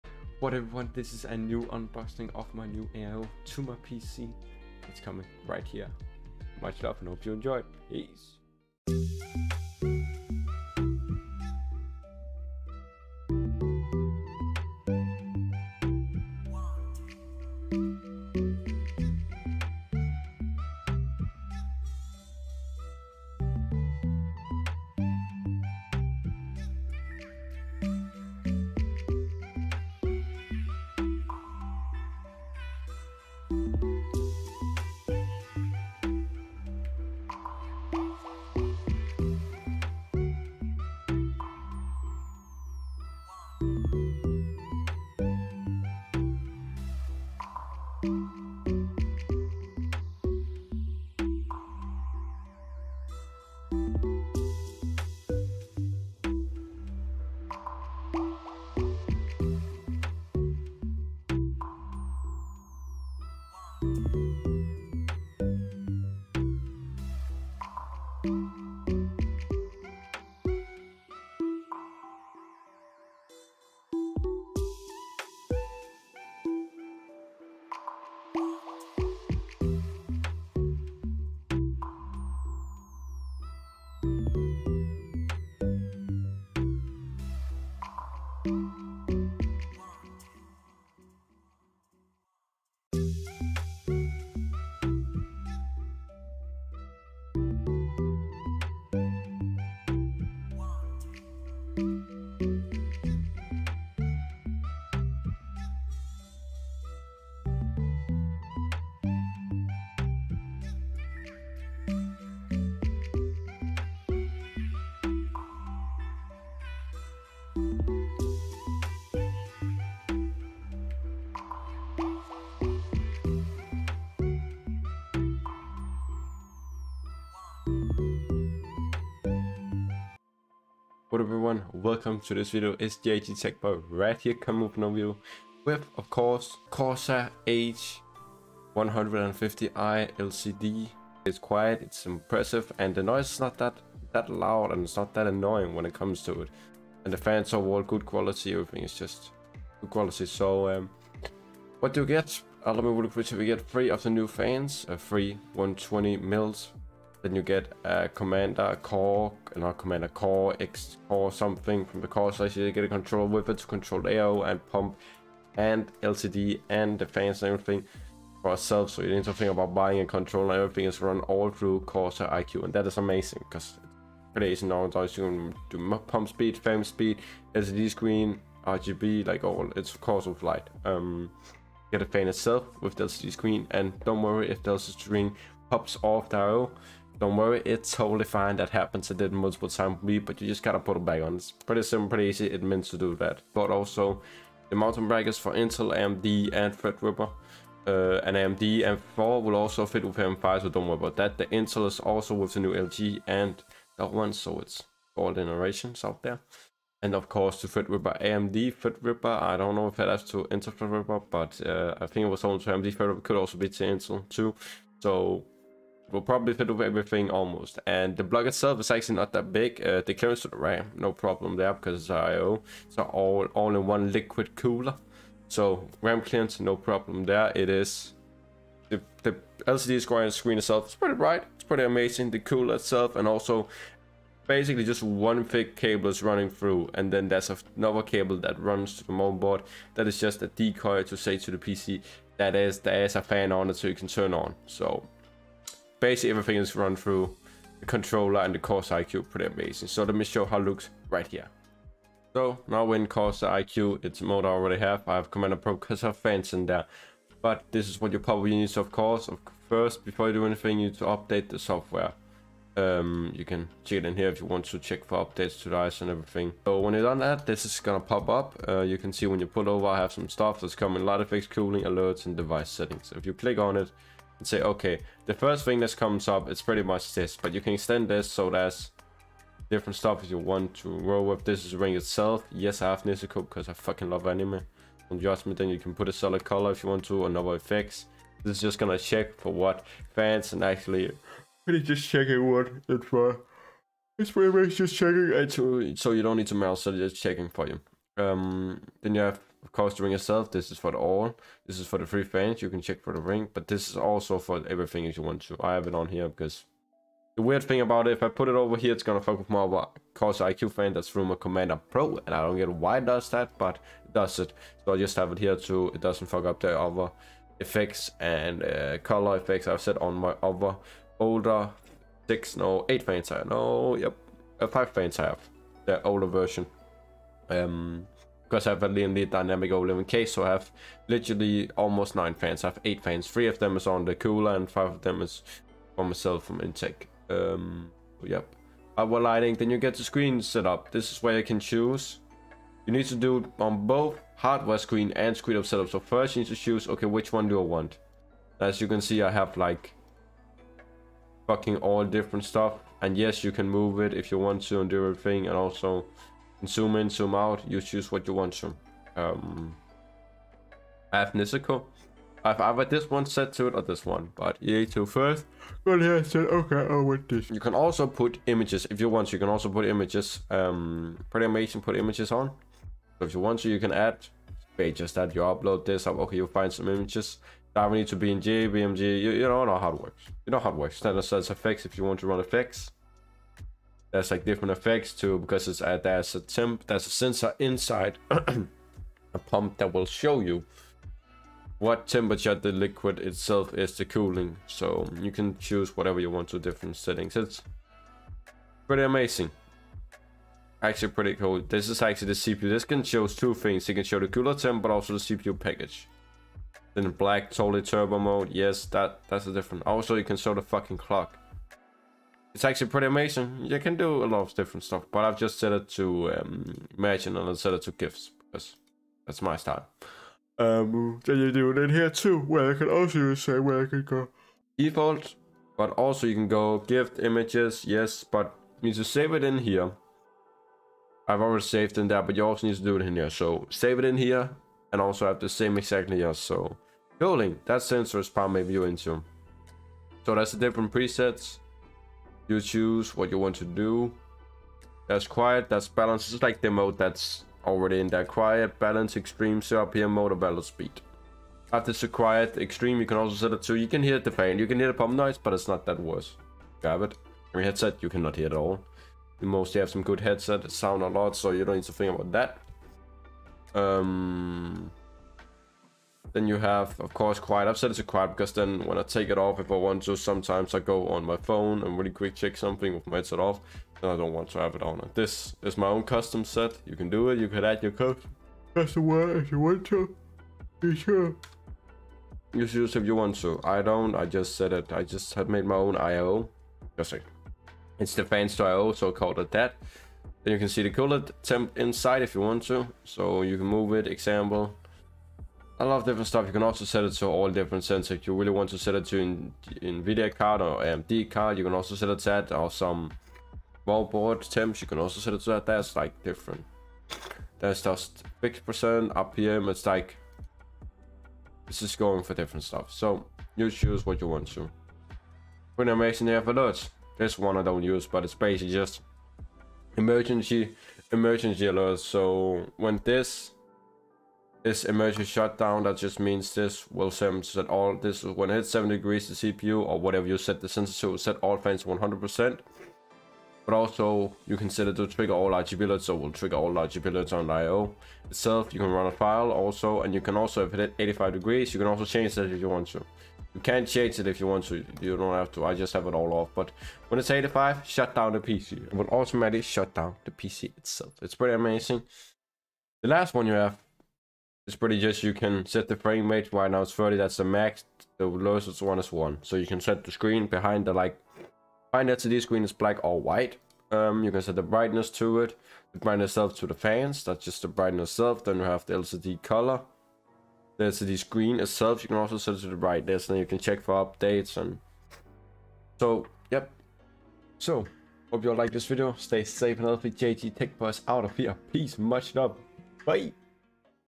Corsair H150I ELITE LCD "Unboxing + explaining how it works"
Hey is your favorite Tech boy incoming with another awesome video!!

Link To Corsair H150I LCD